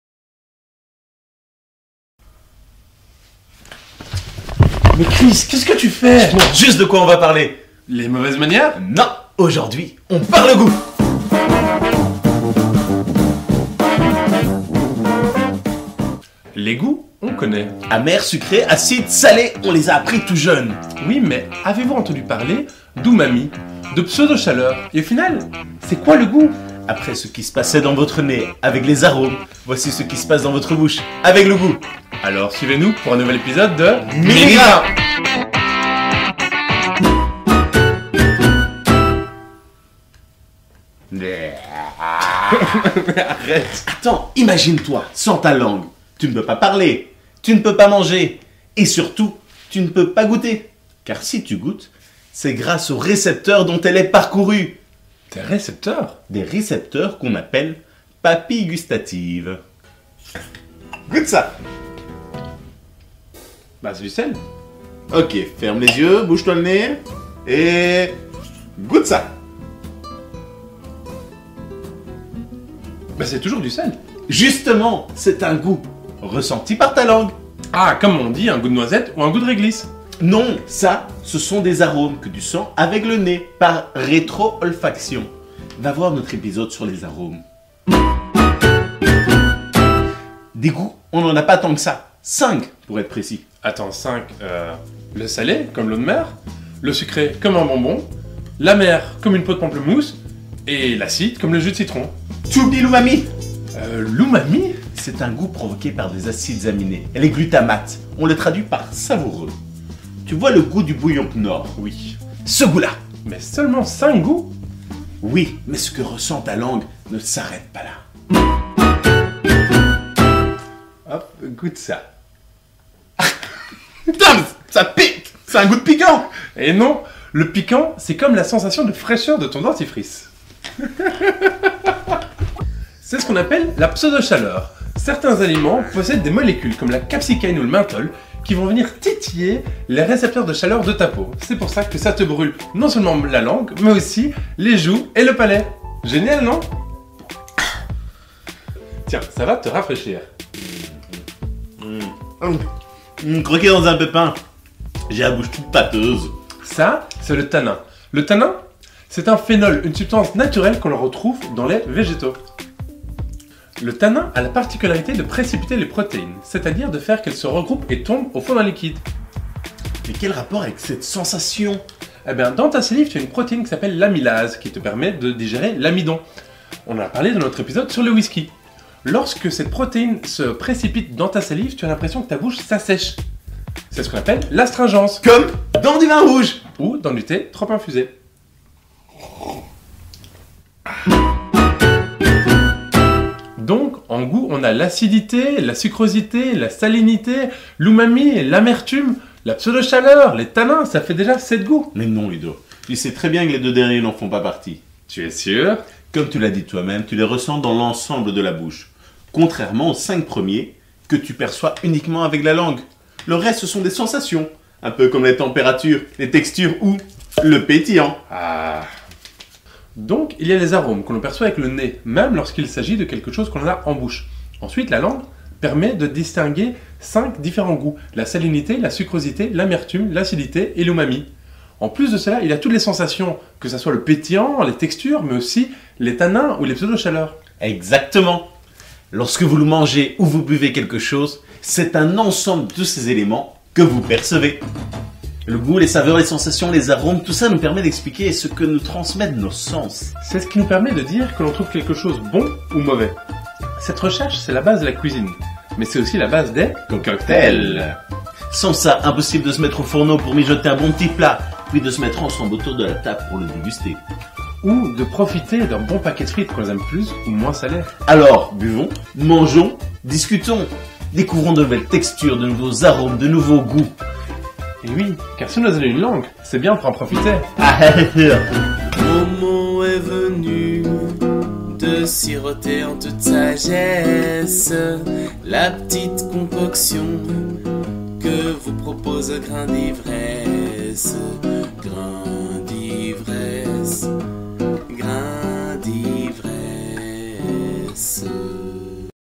C'EST QUOI LE GOÛT ? – MINIGRAIN #03 (sous-titré) | Grain d'Ivresse
Salé, sucré ? Umami ? Après les arômes et ce qui se passait dans votre nez, Grain d'Ivresse vous parle cette fois du GOÛT !